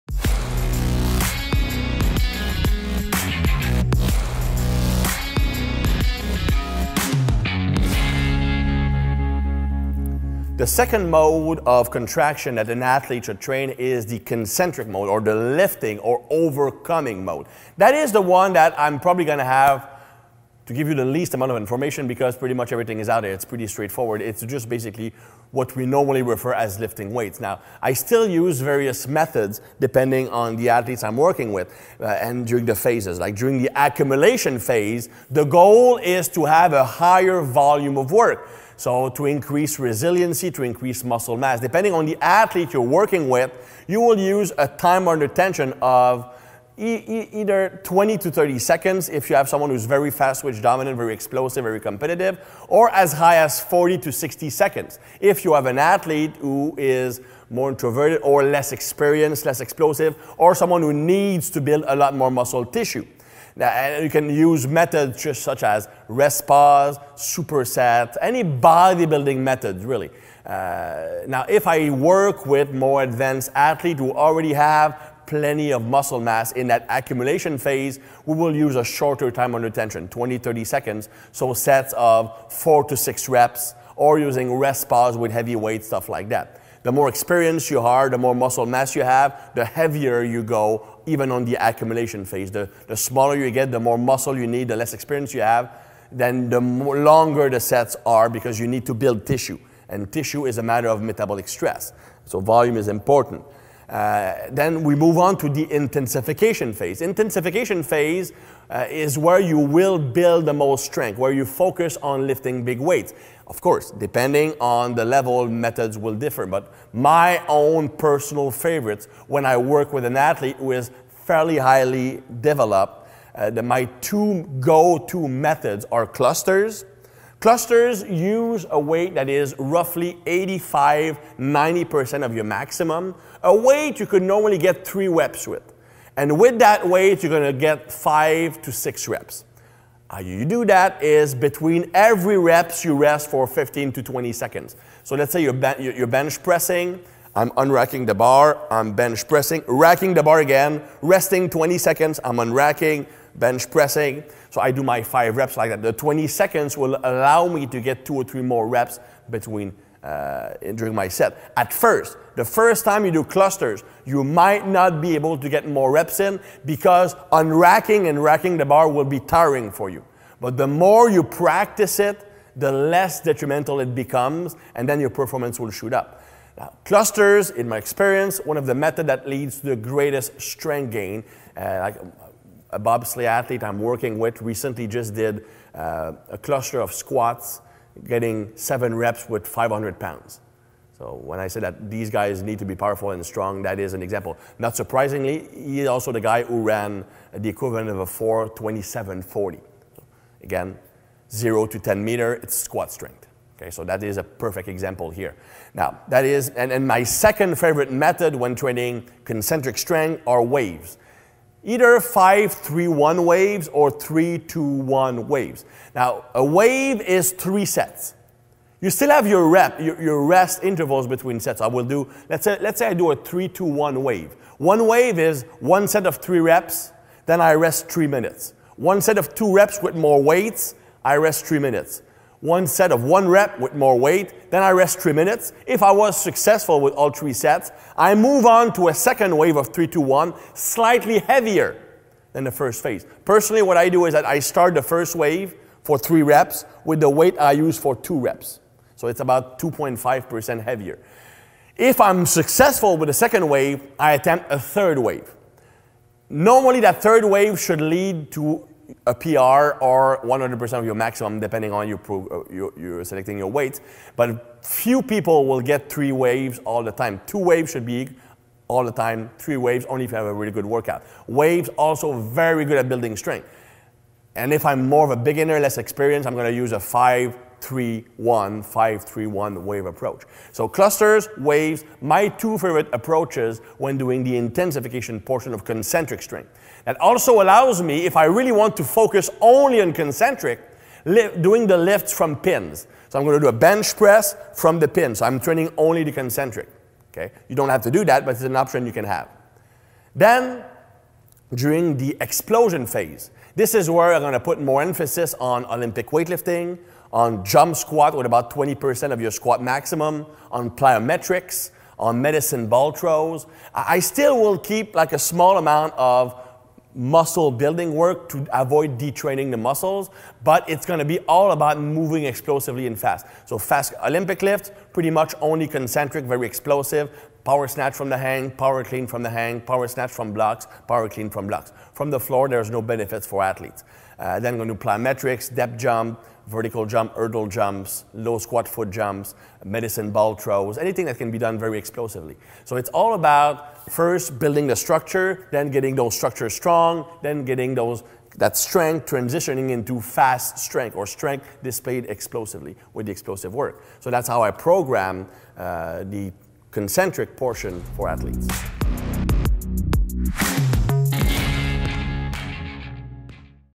PERFORMANCE PRINCIPLES - PART 3
Concentric methods are also important in periodization, otherwise known as the lifting or overcoming mode.   In this capsule, Chris discusses how to use various concentric methods in different phases and for different types of athletes.  Listen now!

Thibarmy - @thibarmy